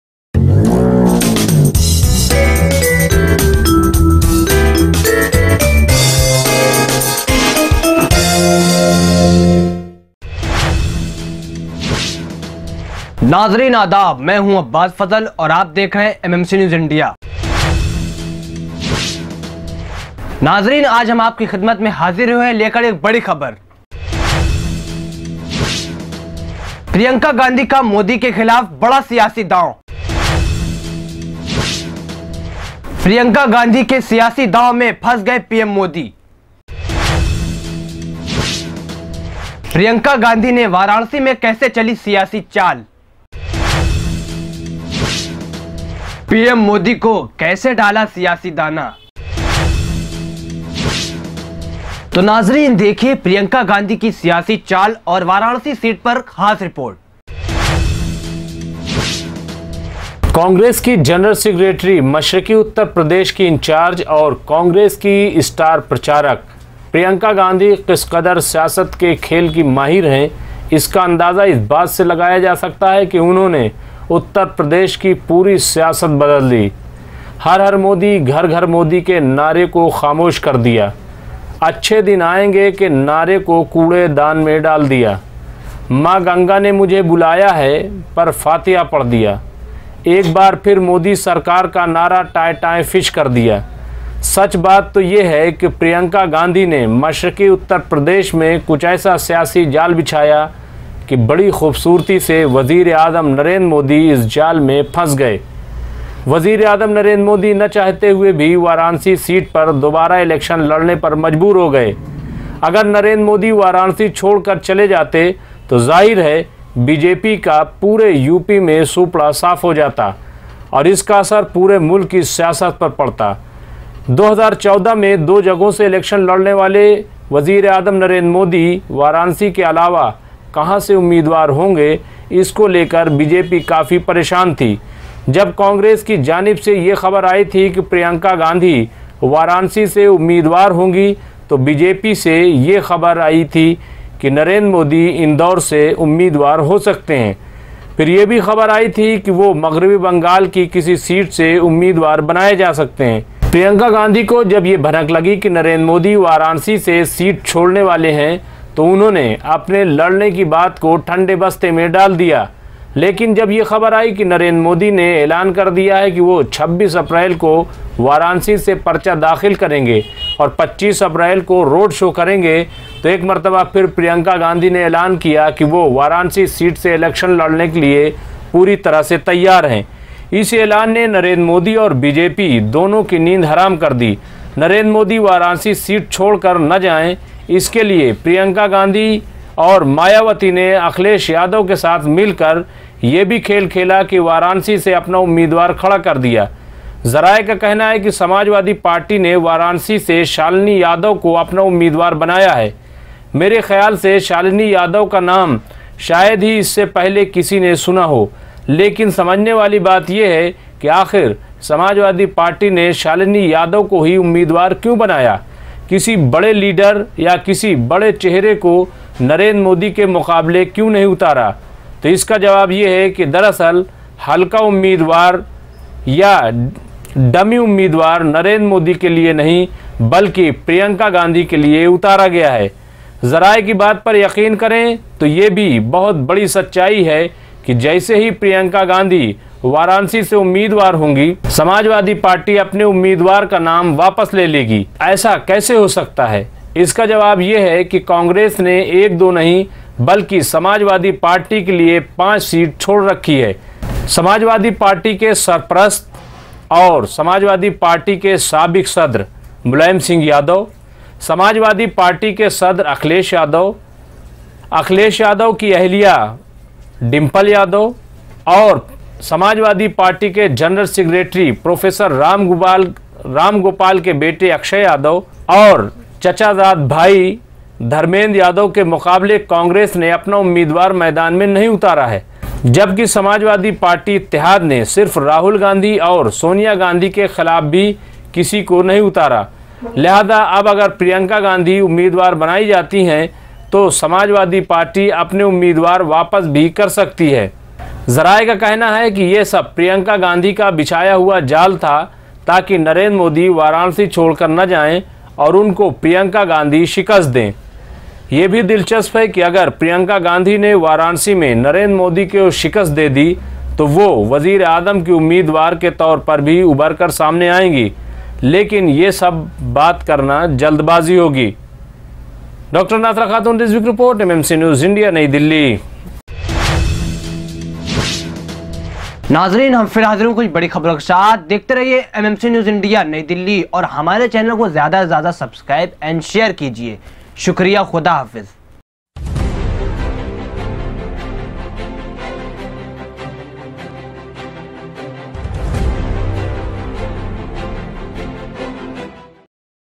Priyanka Gandhi ke jaal me phans gaye pm modi, dekhiye kaise ? || MINORITY MEDIA CENTER
वाराणसी लोक सभा सीट पर जारी सियासत पर देखिये ख़ास रिपोर्ट